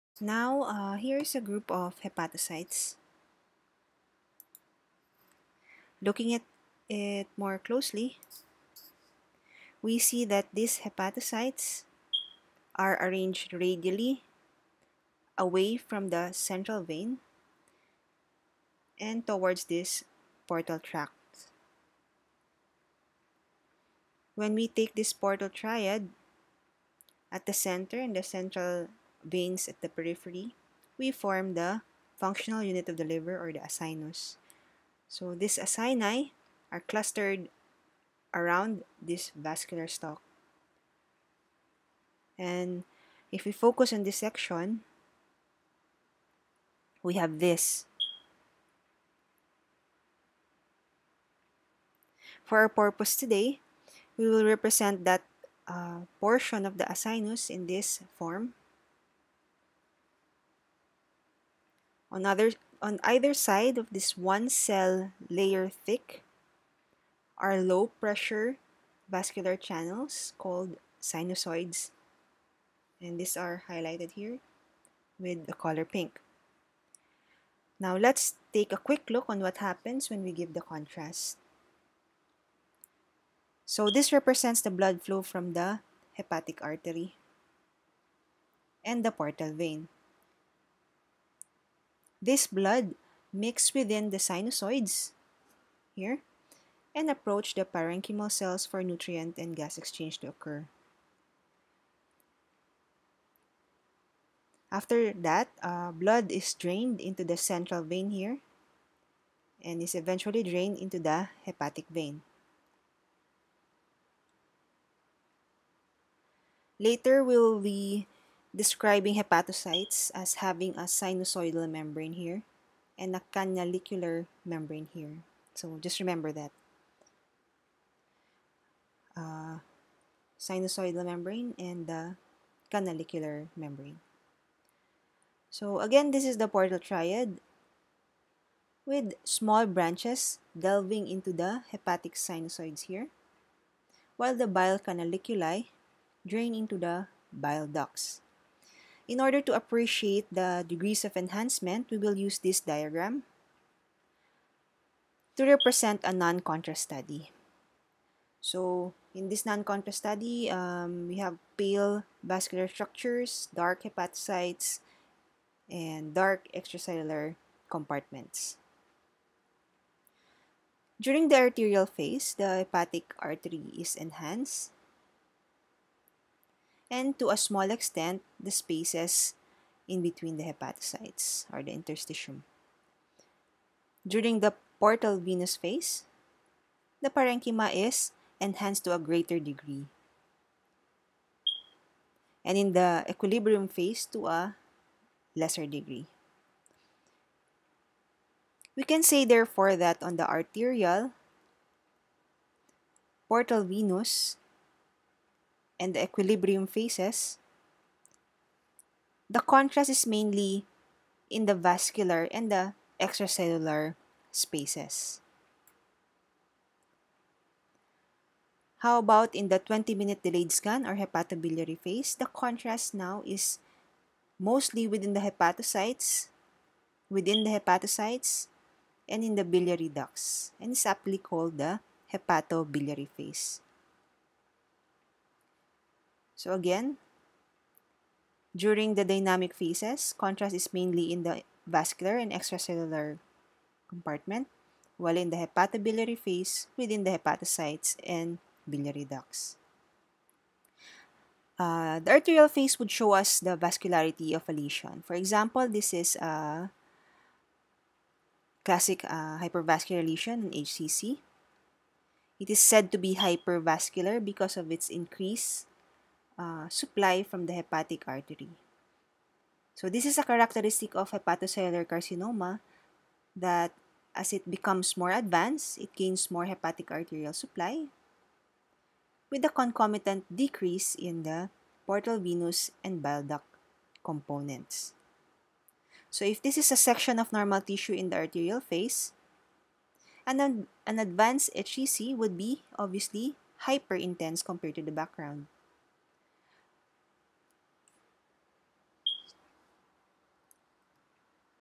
GIT - Basics MR liver-specific contrast (2/5) - Primovist, Eovist : simplified
Simplified explanation on the imaging appearance of lesions in the dynamic and hepatobiliary phase.